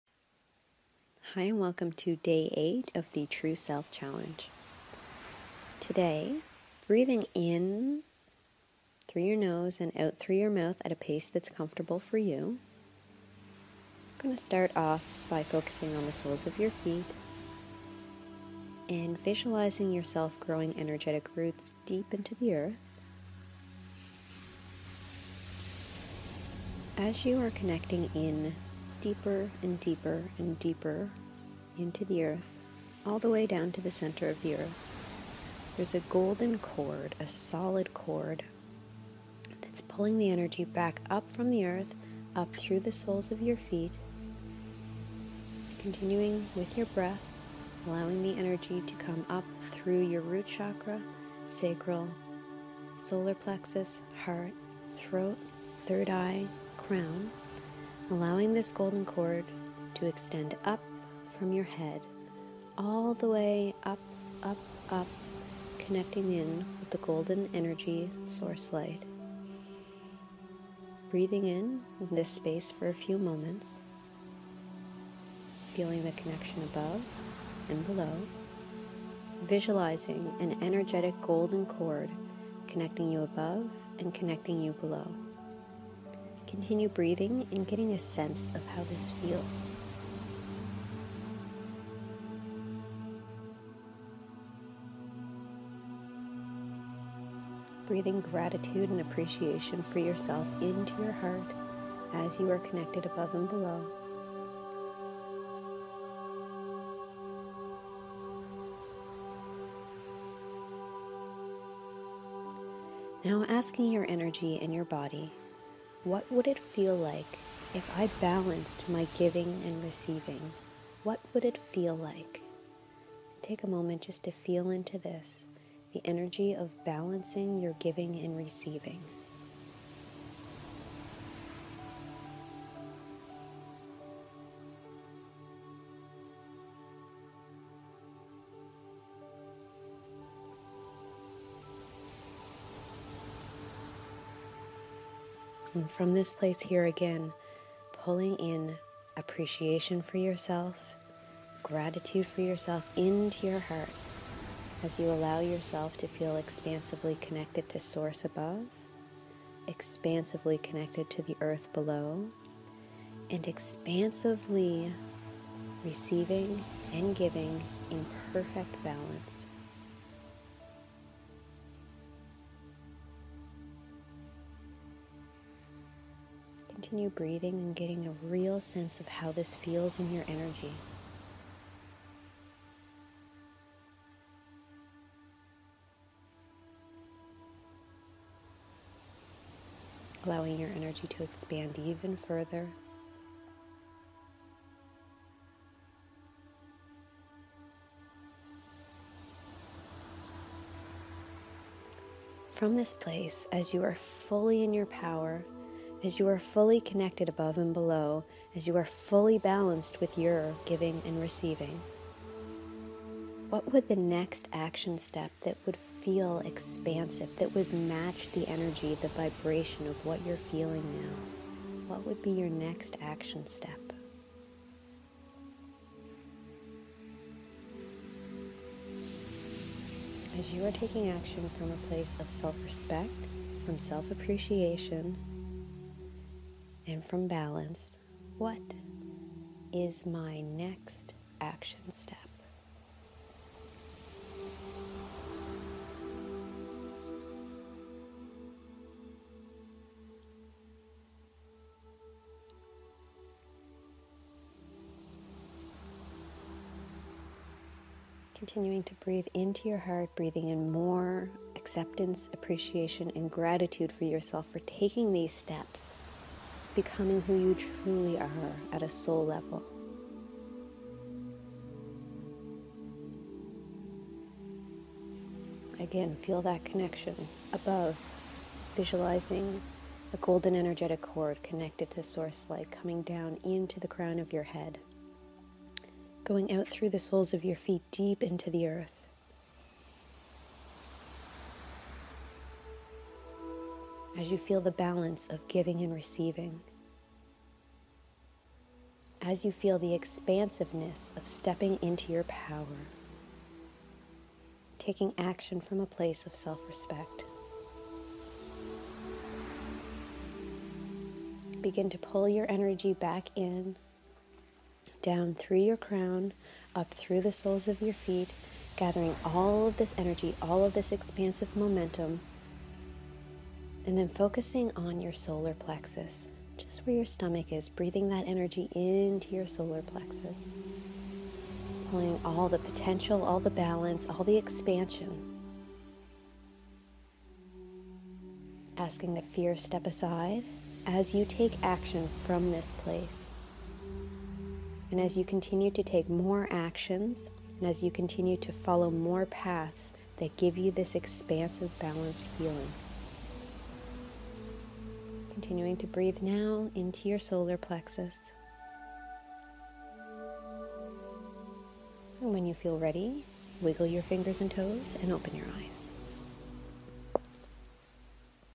Day 8 True Self Challenge
A 10 Day Challenge to believe in yourself, step into your power and take aligned actions. When we believe in ourselves and take actions from a place of personal power we can flow deliberately towards the things we most want to create in this life. To join visit tracygaudet.com